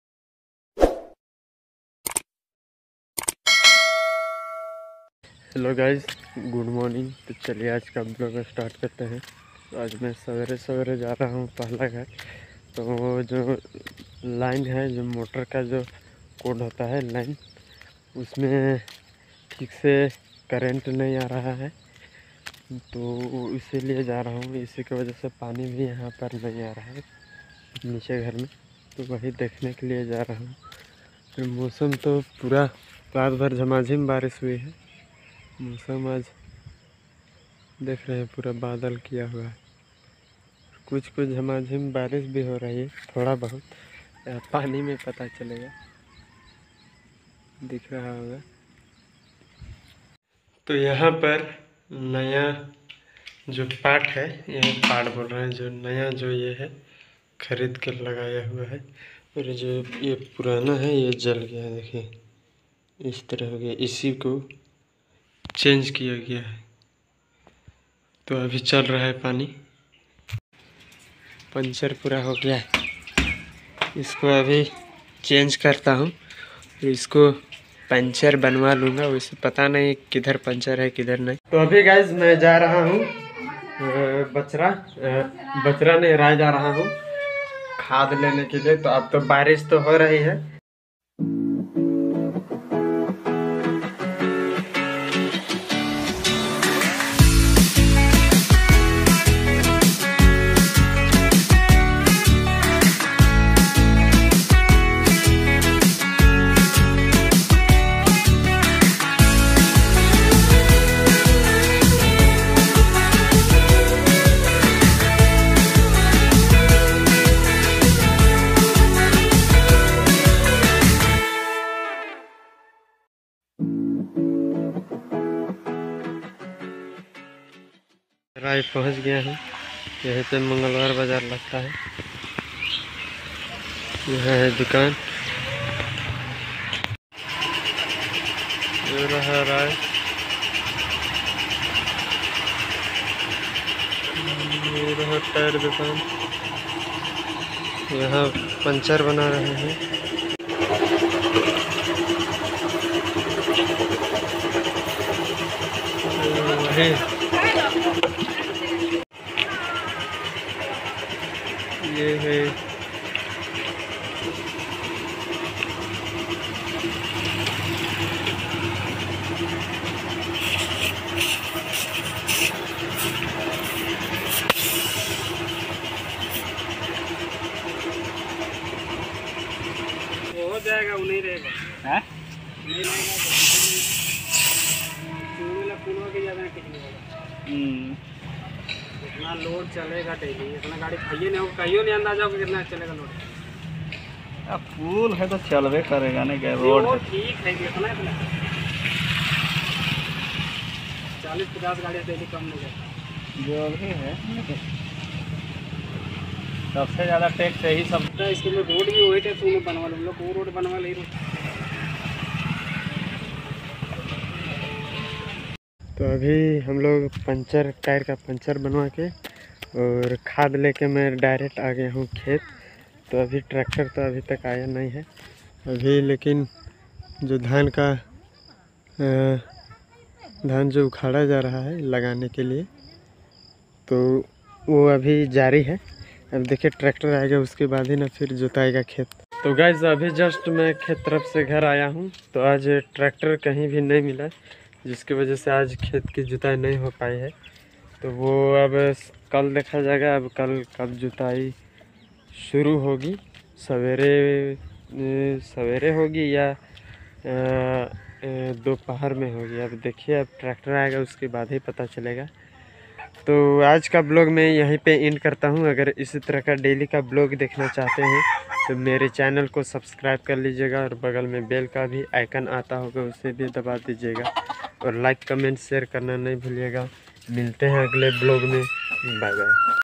Tyre Panchar Ho Gaya
Hello guys aaj main apke liye ek video laya hu, aaj main Ray gaya tha Khad lane aur Gadi ka Tyre Panchar Ho Gaya tha usko bhi Banwa liya hu to video par bane rahe aur Like comment and share karna nahi bhuliyega so thank you very much bye bye.